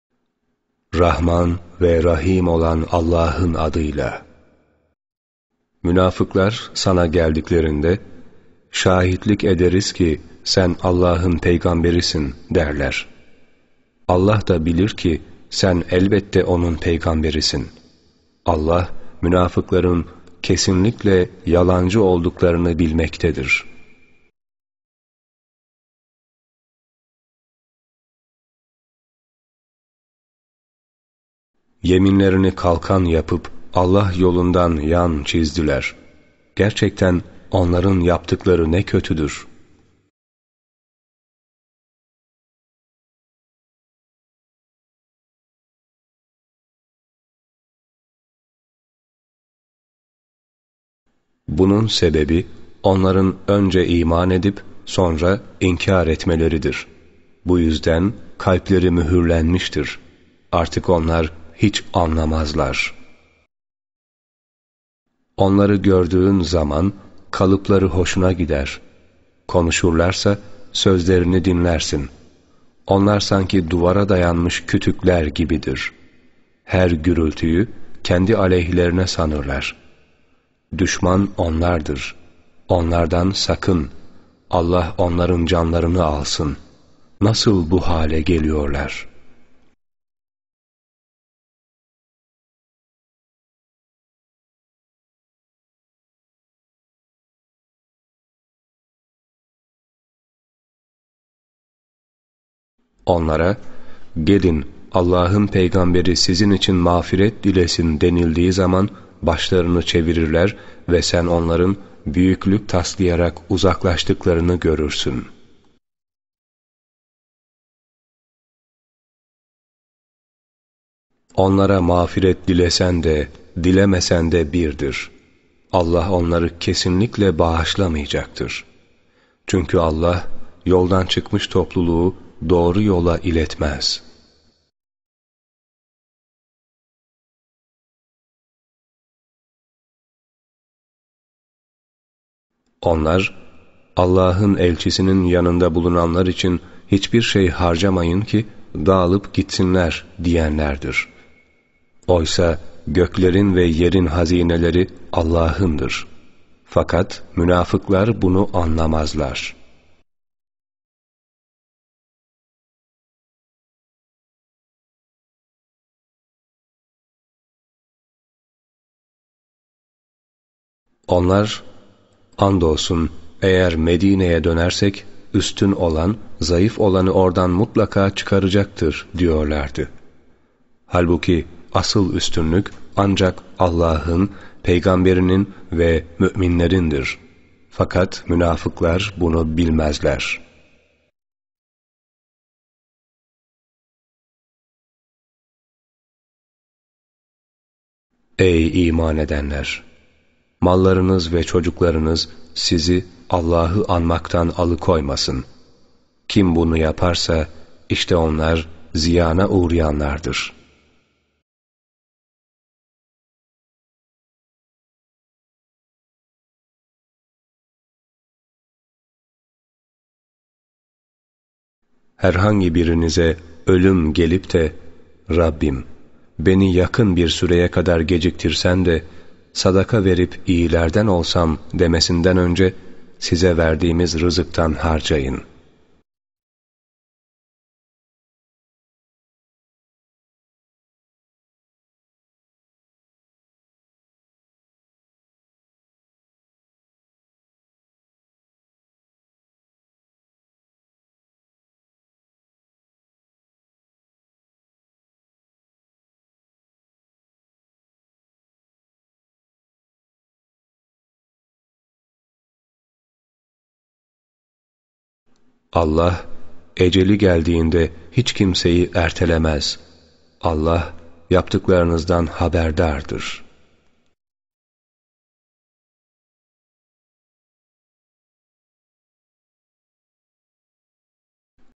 Münafikun Suresi - Türkçe Meali
Okuyan : Dr.Fatih Çollak
Hat : Hamit Aytaç
Meal Seslendirme : Seyfullah Kartal

Dr.Fatih Çollak Hoca'nın muhteşem kıraatı ve Hamit Aytaç hattı ile Kur'an-ı Kerim'in hem Arapça hem Türkçe seslendirmesini dinleyebilir aynı zamanda Arapça hattan okla takip edebilirsiniz.

Kur'an-ı anlamak , manasını öğrenmek için muhteşem bir set...
Meali ise Diyanet İşleri Başkanlığı'na ait olup , ekrandan takip edilmektedir.